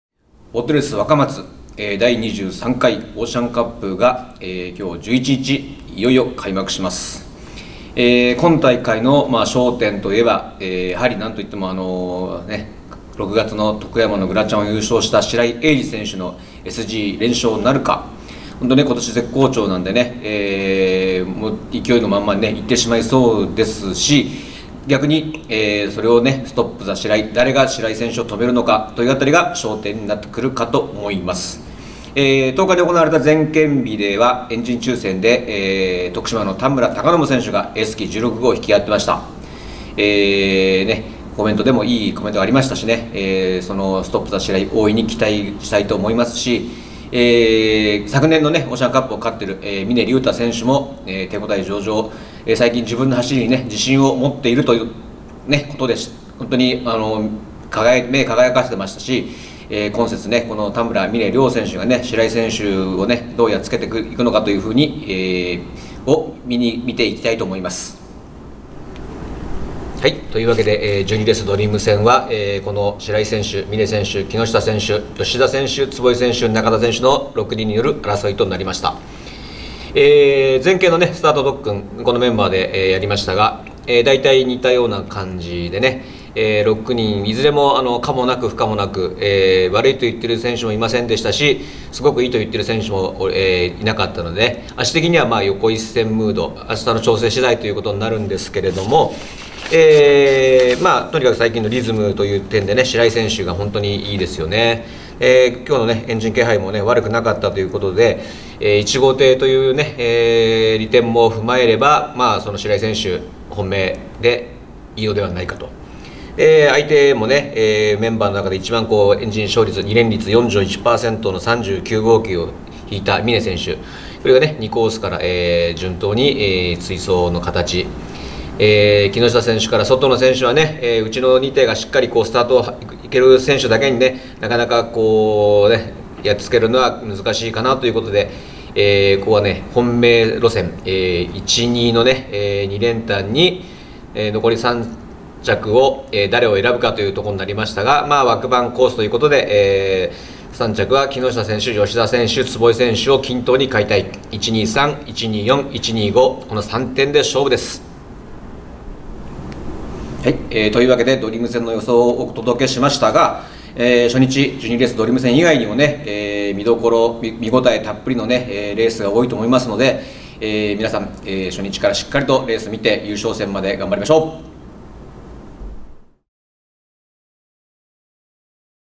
【ＳＧオーシャンカップ】本紙記者の初日（１１日）ドリーム戦予想
１１日にボートレース若松で開幕する「ＳＧオーシャンカップ」。本紙・福原健記者の初日ドリーム戦予想をお届けします。